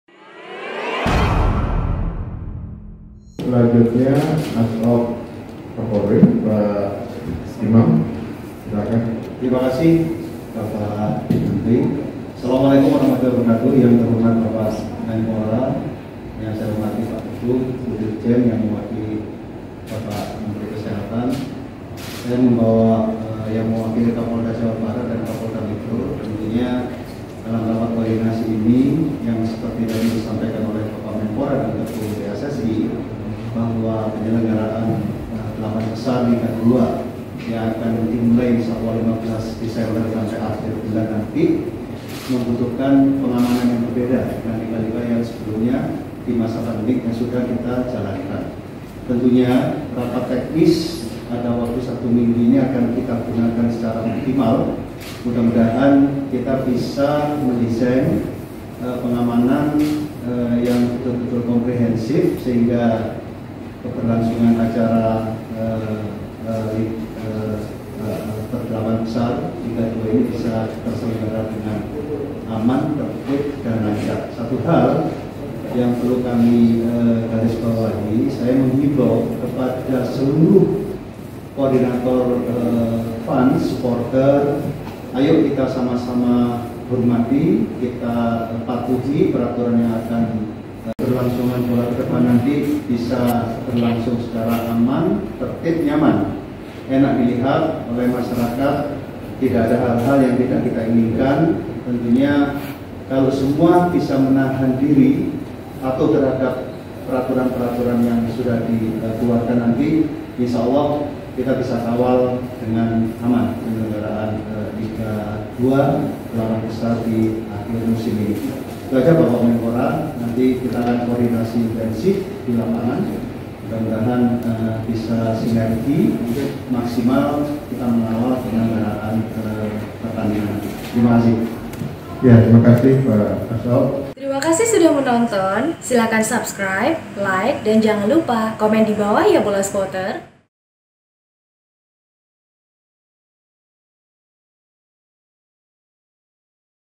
Uji Coba Penonton Liga 2 Fix Dimulai, Polri Siapkan Pengamanan Berbeda
Uji coba menghadirkan penonton di babak delapan besar Liga 2 2020 akan dilansungkan pada 15 Desember mendatang di Stadion Pakansari dan Wibawa Mukti.

Asisten Operasi Kapolri, Irjen Pol Drs. Imam Sugianto mengatakan bahwa pihaknya akan menerapkan pengamanan yang berbeda dari babak penyisihan grup yang dilangsungkan tanpa penonton.

Lebih lanjut, Imam juga meminta agar para pentolan suporter bisa bekerja sama dengan menghormati aturan yang ada nantinya agar tidak ada hal-hal yang tak diinginkan.

Follow & Mention Twitter kami:
Like & Follow Facebook:
BolaSport.com